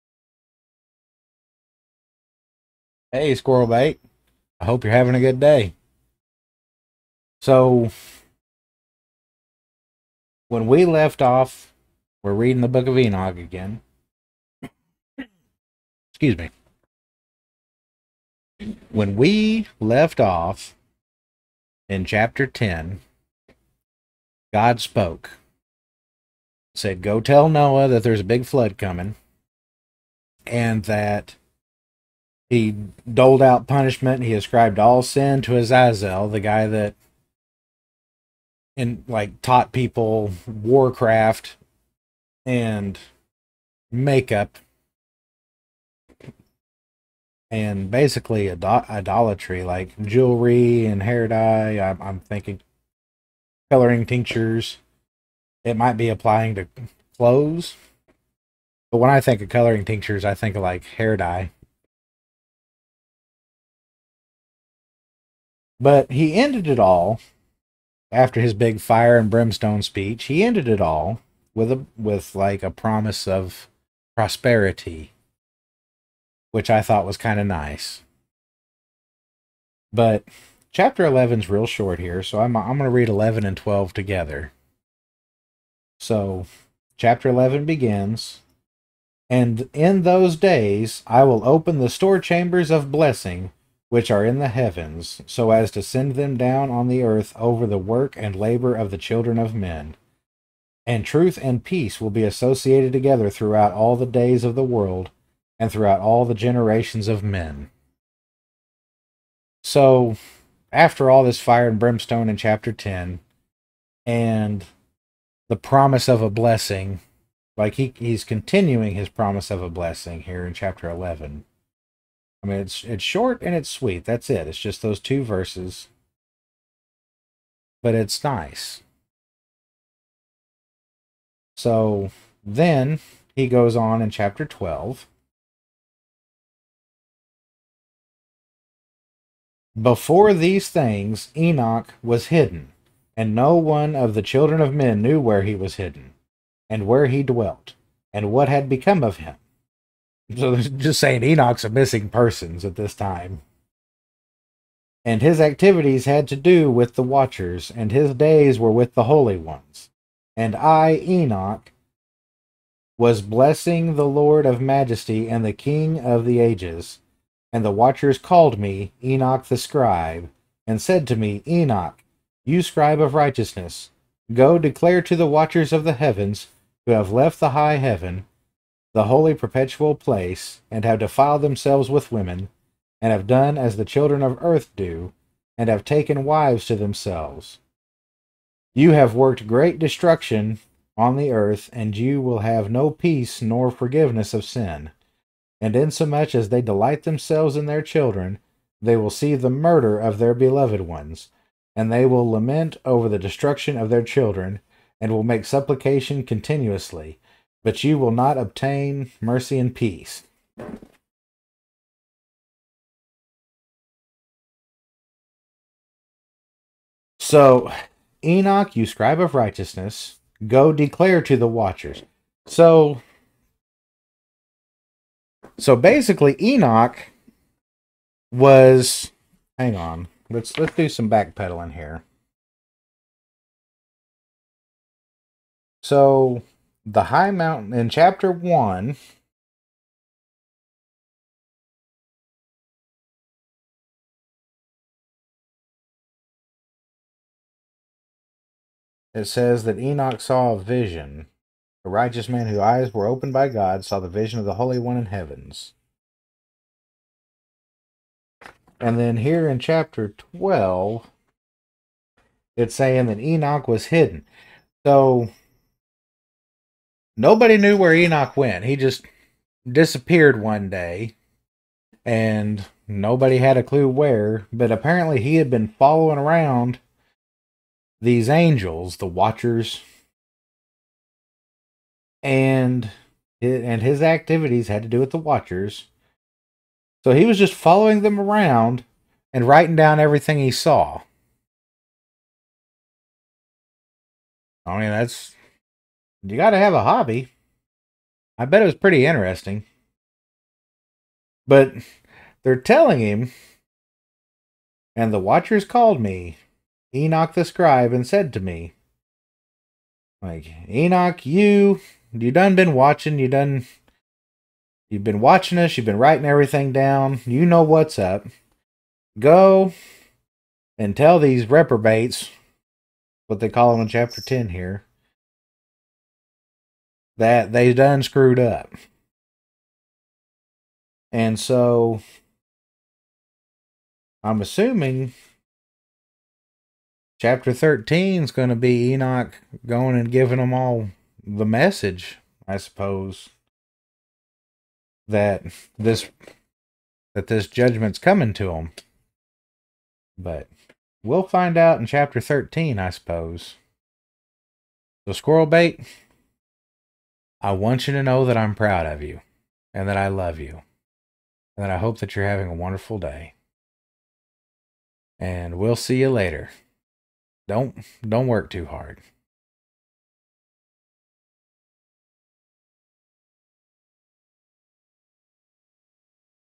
The Book of Enoch Chapter 11 and 12 (Enoch is commanded to deliver a message)
enoch the missing persons is ordered by the remaining watchers to deliver a message to the reprobates.